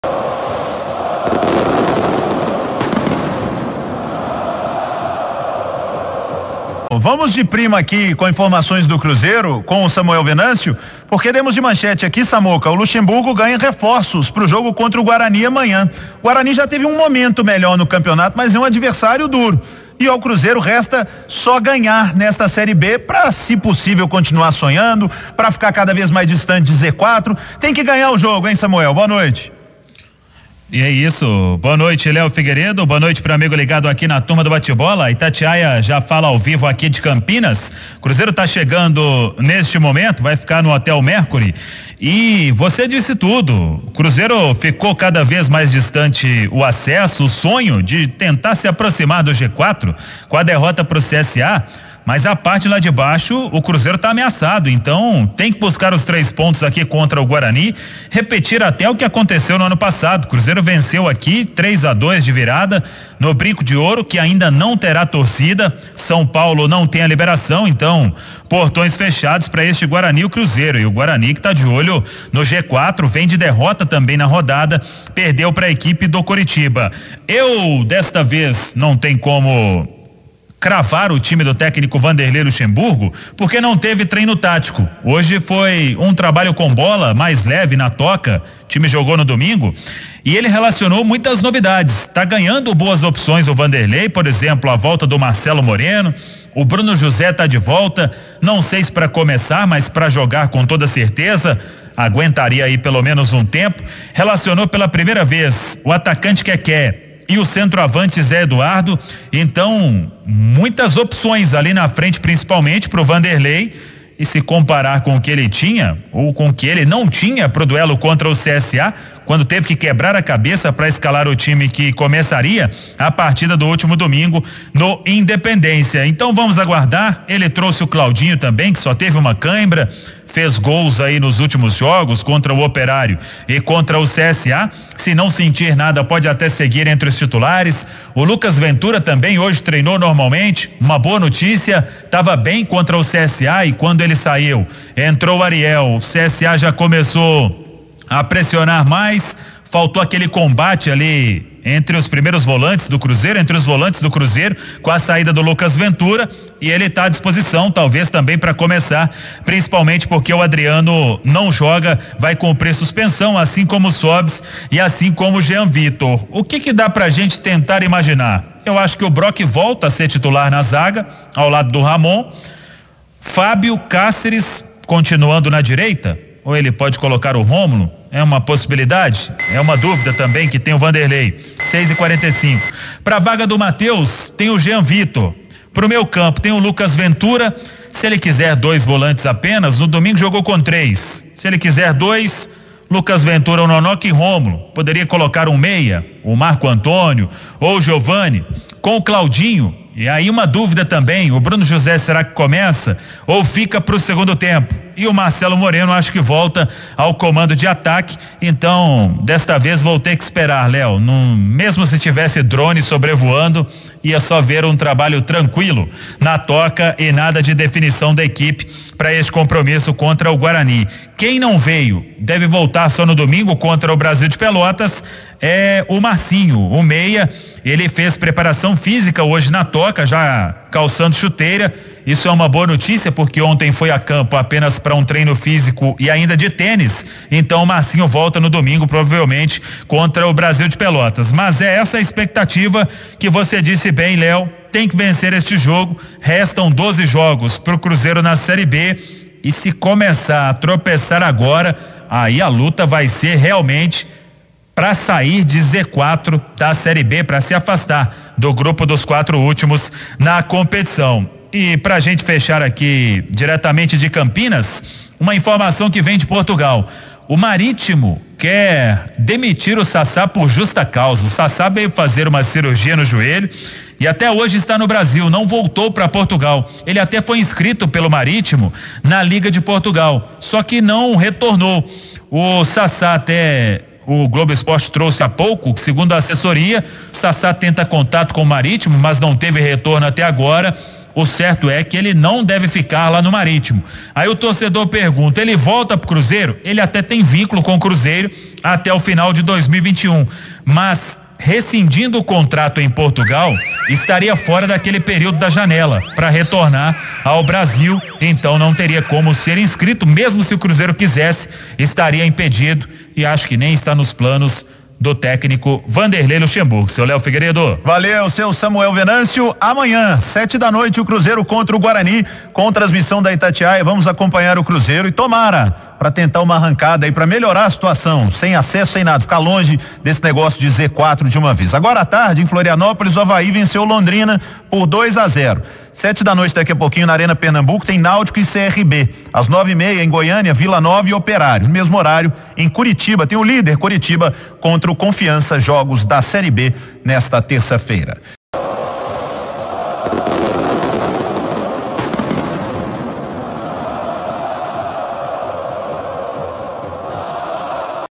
NOTICIÁRIO DO CRUZEIRO EDIÇÃO NOITE | MARCELO MORENO, ZÉ EDUARDO, PROVÁVEL TIME CONTRA O GUARANI
QUAL TIME LUXA VAI ESCALAR CONTRA O GUARANI?